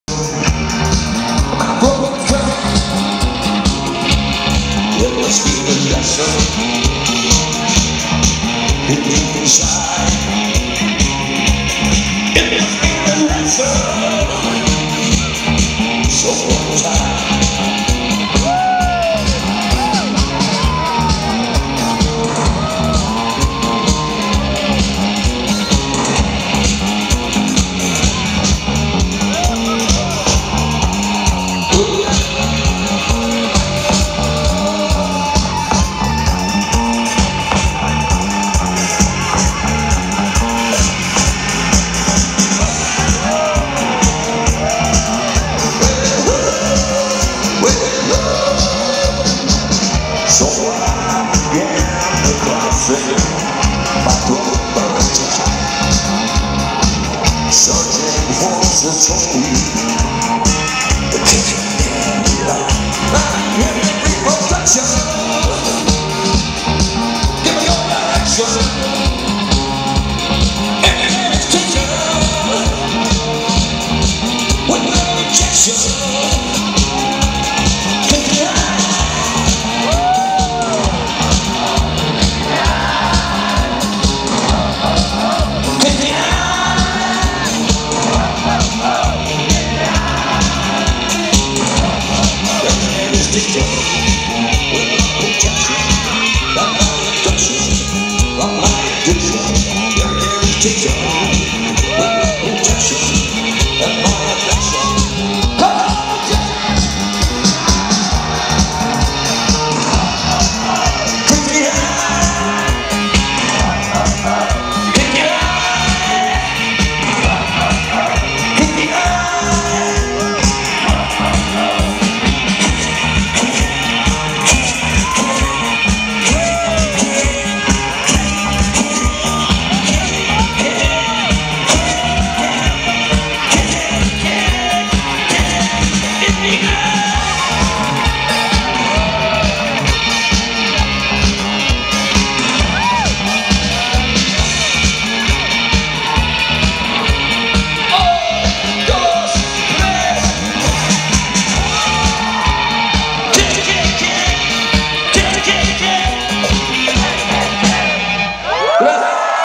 Bauhaus Reunion Tour 2021 / México / Parque Bicentenario
La gira de Bauhaus en Ciudad de México, 23 y 24 de octubre en el Parque Bicentenario. Reseña en la siguiente liga.